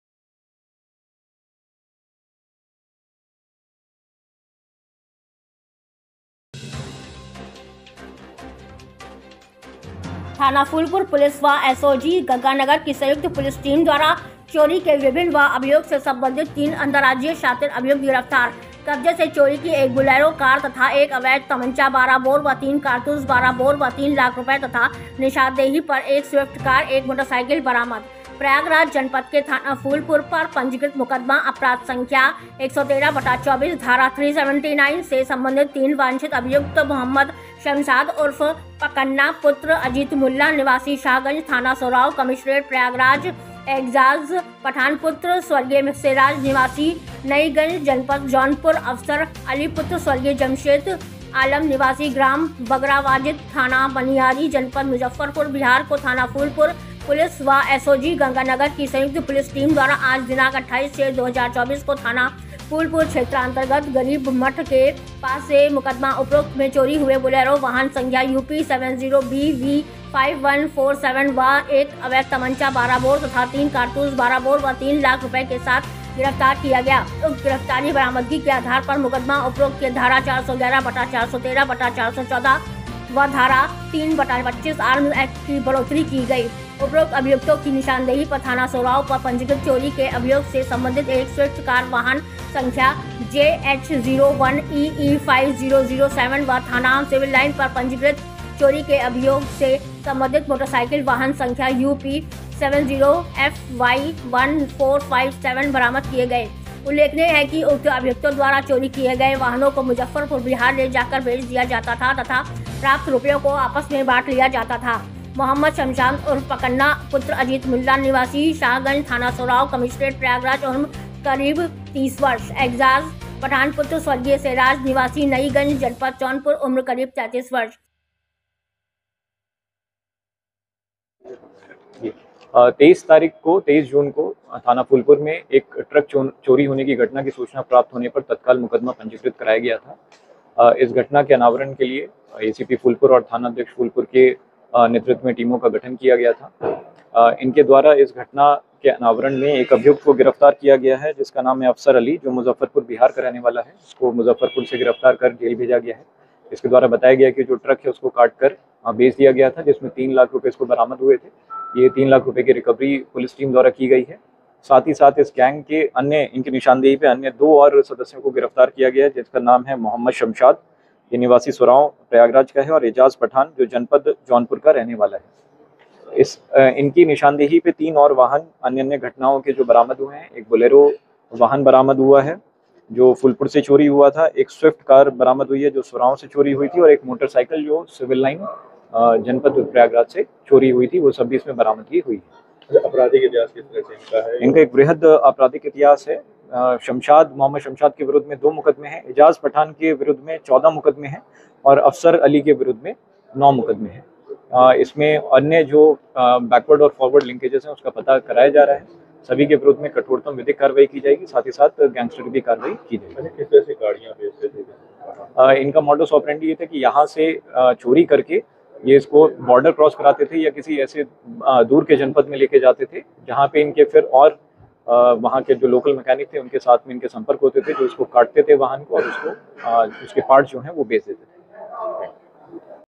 थाना फूलपुर पुलिस व एस0ओ0जी0 गंगानगर की संयुक्त पुलिस टीम को मिली सफलता
*थाना फूलपुर पुलिस व एस0ओ0जी0 गंगानगर की संयुक्त पुलिस टीम द्वारा चोरी के विभिन्न अभियोग से सम्बंधित 03 अन्तर्राज्यीय शातिर अभियुक्त गिरफ्तार, कब्जे से चोरी की 01 बोलेरो कार तथा 01 अवैध तमंचा .12 बोर व 03 कारतूस .12 बोर व 03 लाख रूपये तथा निशादेही पर 01 स्विफ्ट कार, 01 मोटर साईकिल बरामद*

*संवाददाता अजय पांडे थाना फूलपुर प्रयागराज*

प्रयागराज जनपद के थाना फूलपुर पर पंजीकृत मु0अ0सं0- 113/24 धारा-379 भा0द0सं0 से सम्बंधित 03 वांछित अभियुक्त 1.मो0 शमसाद उर्फ पकन्ना पुत्र अजीतमुल्ला निवासी शाहगंज थाना सोरांव कमिश्नरेट प्रयागराज 2. एजाज पठान पुत्र स्व0 सेराज निवासी नईगंज जनपद जौनपुर 3. अफसर अली पुत्र स्व0 जमशेद आलम निवासी ग्राम बगरावाजित थाना मनियारी जनपद मुजफ्फरपुर बिहार को थाना फूलपुर पुलिस व एस0ओ0जी0 गंगानगर की संयुक्त पुलिस टीम द्वारा आज दिनांक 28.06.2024 को थाना फूलपुर क्षेत्रान्तर्गत करीबमठ के पास से मुकदमा उपरोक्त मे चोरी हुए बोलेरो वाहन सं0- UP70BV5147 व 01 अवैध तमंचा .12 बोर तथा 03 कारतूस .12 बोर व 03 लाख रूपये के साथ गिरफ्तार किया गया । उक्त गिरफ्तारी/बरामदगी के अधार पर मुकदमा उपरोक्त मे धारा